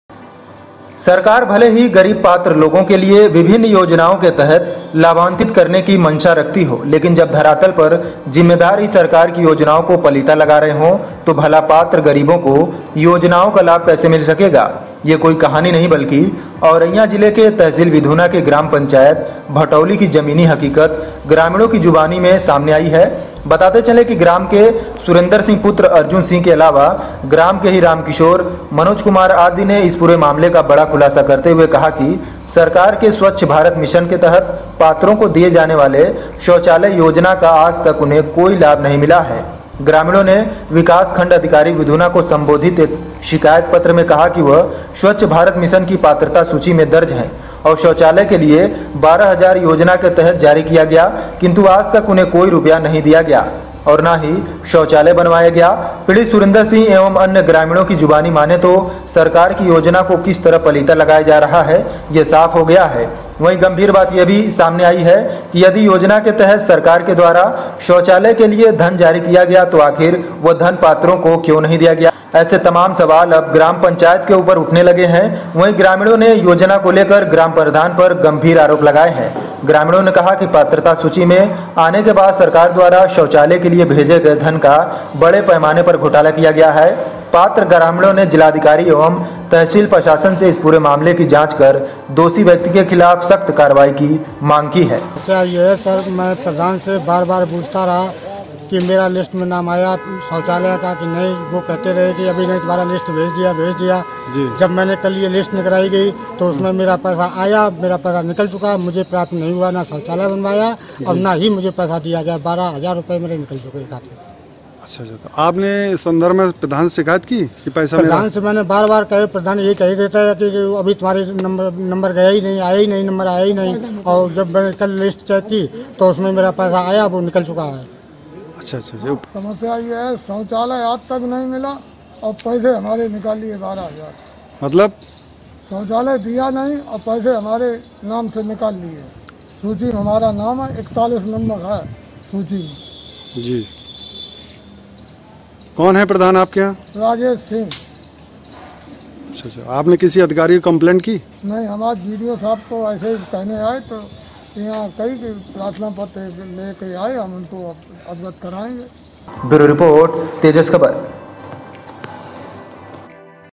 AURAIYA: ग्रामीणों को कागजों पर शौचालय का धन मिला पर हकीकत में नही...
बिधूना। सरकार भले ही गरीब पात्र लोगों के लिए विभिन्न योजनाओं के तहत लाभान्वित करने की मंशा रखती हो, लेकिन जब धरातल पर जिम्मेदार ही सरकार की योजनाओं को पलीता लगा रहे हो तो भला पात्र गरीबों को योजनाओं का लाभ कैसे मिल सकेगा। यह कोई कहानी नहीं बल्कि औरैया जिले की तहसील बिधूना की ग्राम पंचायत भटौली की जमीनी हकीकत ग्रामीणों की जुबानी में सामने आई है।  बताते चलें की ग्राम के सुरेंद्र सिंह पुत्र अर्जुन सिंह के अलावा ग्राम के ही रामकिशोर, मनोज कुमार आदि ने इस पूरे मामले का बड़ा खुलासा करते हुए कहा कि सरकार के स्वच्छ भारत मिशन के तहत पात्रों को दिए जाने वाले शौचालय योजना का आजतक उन्हें कोई लाभ नही मिला। ग्रामीणों ने विकास खंड अधिकारी बिधूना को संबोधित एक शिकायत पत्र में कहा कि वह स्वच्छ भारत मिशन की पात्रता सूची में दर्ज है और शौचालय के लिए ₹12000 योजना के तहत जारी किया गया किंतु आज तक उन्हें कोई रुपया नहीं दिया गया और ना ही शौचालय बनवाया गया। पीड़ित सुरेन्द्र सिंह एवं अन्य ग्रामीणों की जुबानी माने तो सरकार की योजना को किस तरह पलीता लगाया जा रहा है यह साफ हो गया है। वही गम्भीर बात यह भी सामने आई हैं कि यदि योजना के तहत सरकार के द्वारा शौचालय के लिये धन जारी किया गया तो आखिर वह धन पात्रों को क्यों नही दिया गया। ऐसे तमाम सवाल अब ग्राम पंचायत के ऊपर उठाने लगे हैं। वही ग्रामीणों ने योजना को लेकर ग्राम प्रधान पर गंभीर आरोप लगाए हैं।
 ग्रामीणों ने कहा कि पात्रता सूची में आने के बाद सरकार द्वारा शौचालय के लिए भेजे गये धन का बड़े पैमाने पर घोटाला किया गया है। पात्र ग्रामीणों ने जिलाधिकारी एवं तहसील प्रशासन से इस पूरे मामले की जांच कर दोषी व्यक्ति के खिलाफ सख्त कार्रवाई की मांग की है।